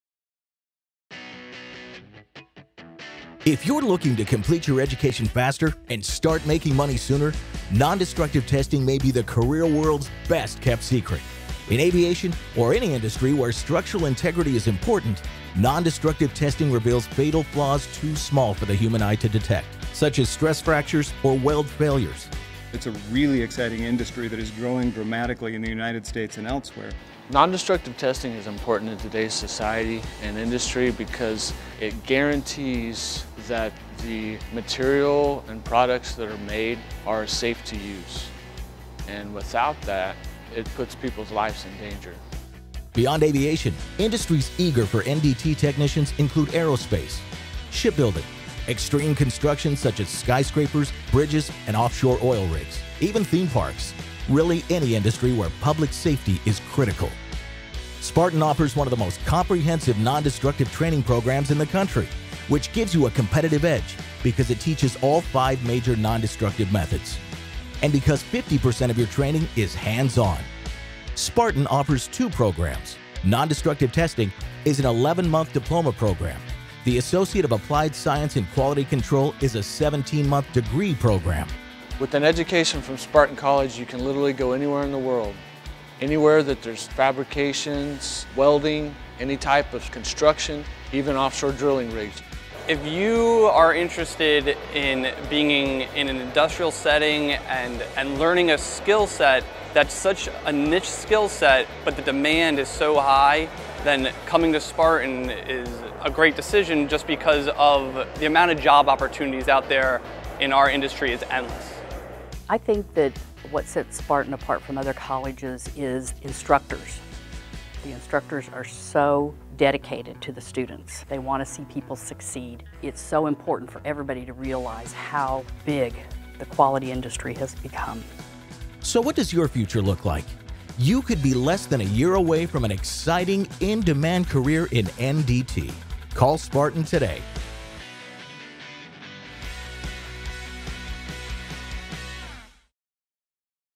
Nondestructive Testing | Spartan College
Our nondestructive testing courses are designed to help you develop the knowledge and skills necessary to perform specialized testing techniques that are required in industries including automotive construction, flight, and energy. Graduates of our NDT testing certification program and Quality Control certification training make very desirable job candidates.